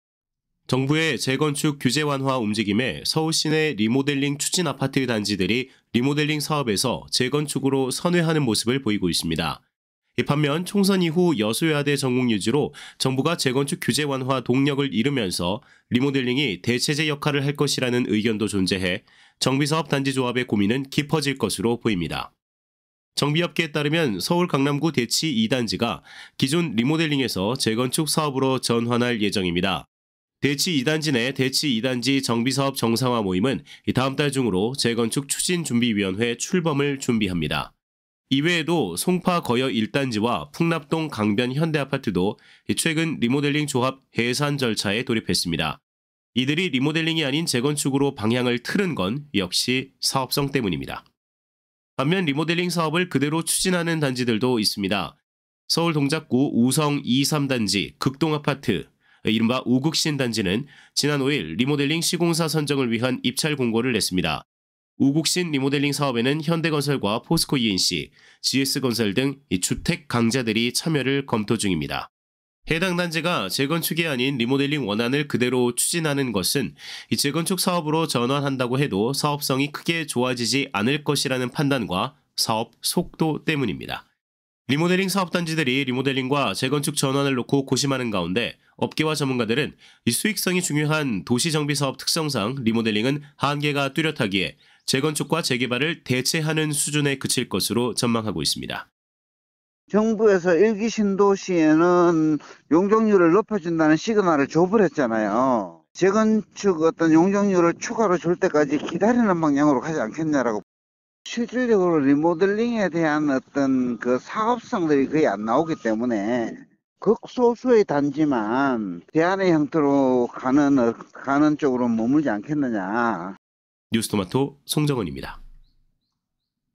리모델링이냐 재건축이냐 [말하는 기자들_산업_0429]
정부의 재건축 규제 완화 움직임에 서울 시내 리모델링 추진 아파트 단지들이 리모델링 사업에서 재건축으로 선회하는 모습을 보이고 있습니다.

반면 총선 이후 여소야대 정국 유지로 정부가 재건축 규제 완화 동력을 잃으면서 리모델링이 대체제 역할을 할 것이라는 의견도 존재해 정비사업 단지 조합의 고민은 깊어질 것으로 보입니다.

정비업계에 따르면 서울 강남구 대치2단지가 기존 리모델링에서 재건축 사업으로 전환할 예정입니다. 대치2단지 내 '대치2단지 정비사업 정상화 모임'은 다음 달 중으로 '재건축추진준비위원회' 출범을 준비합니다.

이외에도 송파 거여1단지와 풍납동 강변현대아파트도 최근 리모델링 조합 해산절차에 돌입했습니다. 이들이 리모델링이 아닌 재건축으로 방향을 틀은 건 역시 사업성 때문입니다.

반면 리모델링 사업을 그대로 추진하는 단지들도 있습니다. 서울 동작구 우성2·3단지·극동 아파, 이른바 우극신 단지는 지난 5일 리모델링 시공사 선정을 위한 입찰공고를 냈습니다. 우극신 리모델링 사업에는 현대건설과 포스코이앤씨, GS건설 등 주택 강자 들이 참여를 검토 중입니다.

해당 단지가 재건축이 아닌 리모델링 원안을 그대로 추진하는 것은 재건축 사업으로 전환한다고 해도 사업성이 크게 좋아지지 않을 것으로 판단과 사업 속도 때문입니다.

리모델링 사업 단지들이 리모델링과 재건축 전환을 놓고 고심하는 가운데 업계와 전문가들은 만 전문가들은 수익성이 중요한 도시정비사업 특성 상 리모델링은 한계가 뚜렷하기에 재건축·재개발을 대체하는 수준에 그칠 것으로 전망하고 있습니다.

서진형 광운대 일반대학원 교수는 "정부에서 1기 신도시에는 용적률을 높여준다는 시그널을 줘버렸잖아요. 때문에 어떤 용적률을 추가로 줄 때까지 기다리는 방향으로 가지 않겠느냐"

"리모델링에 대한 사업성들이 거의 안 나오기 때문에 극소수의 단지만 대안의 형태로 가는 쪽으로 머물지 않겠느냐"

뉴스토마토 송정은입니다.

● 제작진
- 기획: 조문식, 강영관
- 구성 취재: 송정은
- 연출: 송정은

감사합니다.